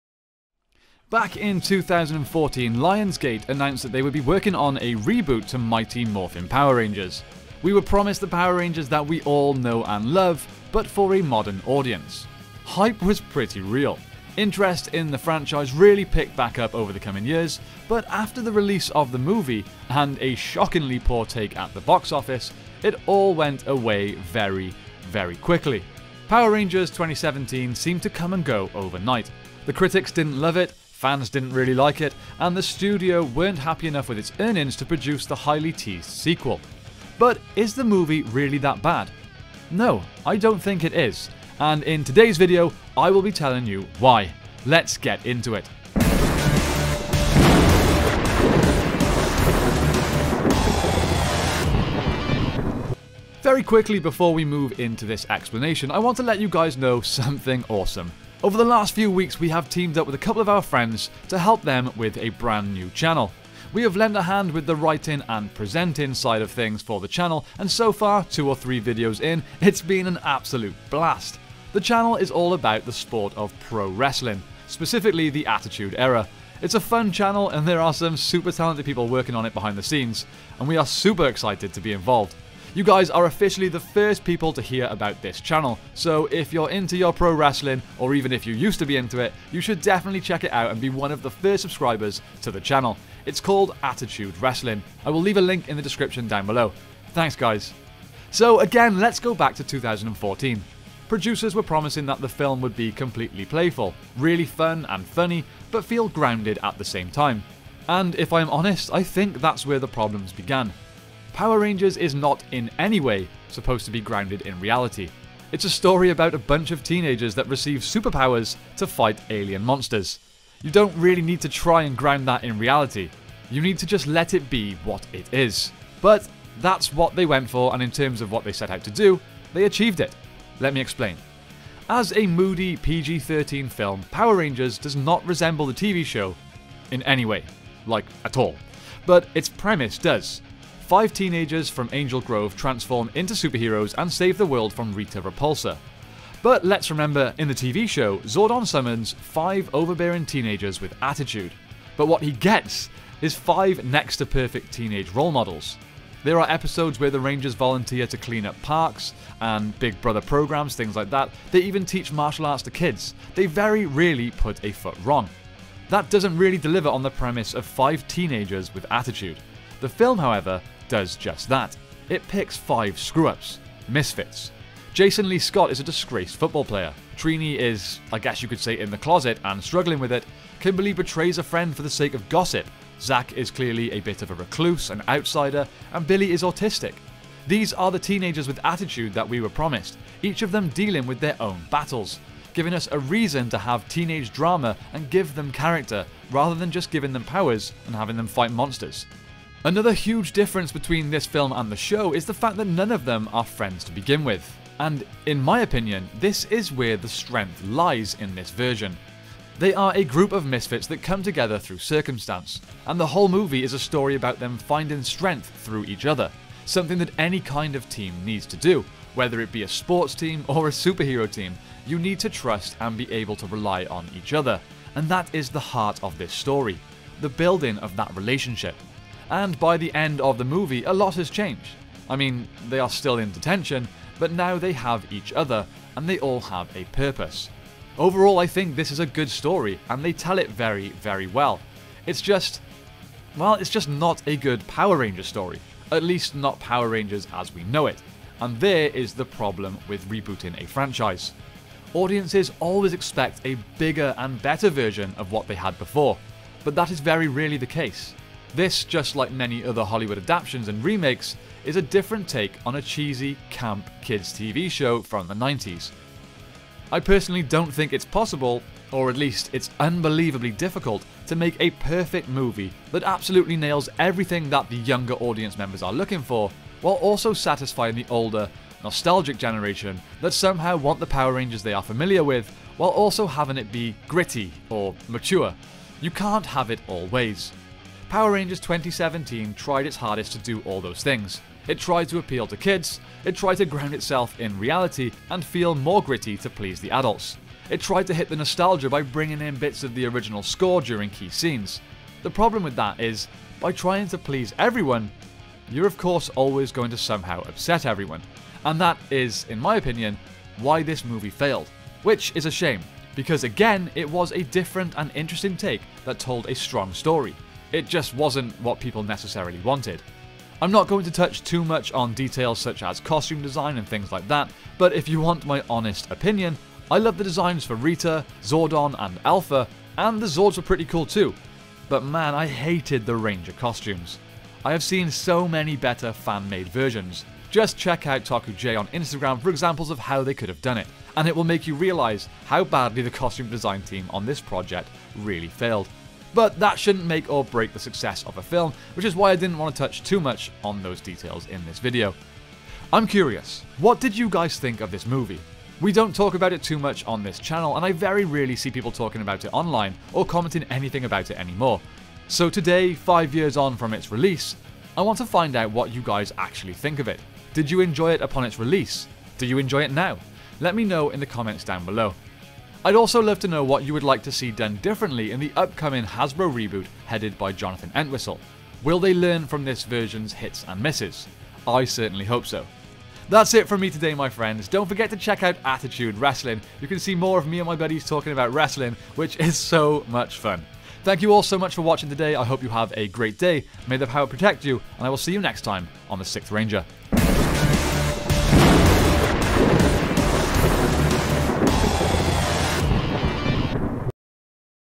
POWER RANGERS (2017) Is Misunderstood!
Today I am talking about an area of Power Rangers that I don't often cover... The 2017 Power Rangers Movie. I hope you enjoy!

Huge thank you to my buddy Bryan for the awesome music you hear on this video. Be sure to check out his channel and show some support!